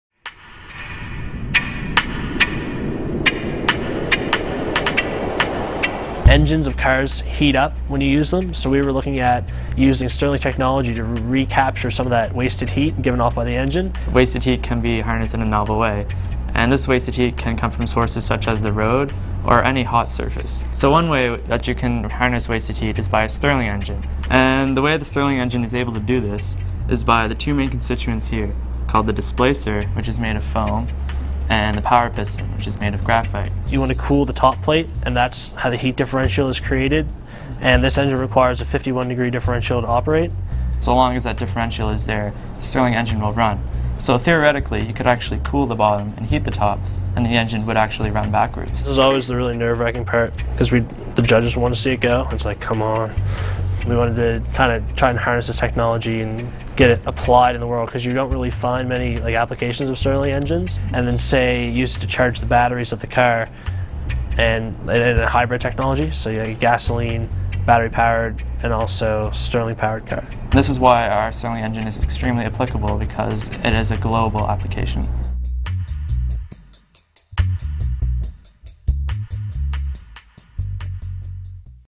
2007 Youth Award - Discovery Awards
"The Economizer" - Oliver Hatheway and Darren Rodenhizer, Park View Education Centre, Bridgewater.
For their science project, Darren and Oliver designed "The Economizer", a sterling engine that converts wasted heat into motion. The two were winners in their school's science fair and went on to win the South Shore Regional science fair. In May of 2007 the pair competed as part of Team Nova Scotia at the Canada Wide Science Fair where they brought home an honourable mention in the automotive category and the Petro-Canada Innovation Award.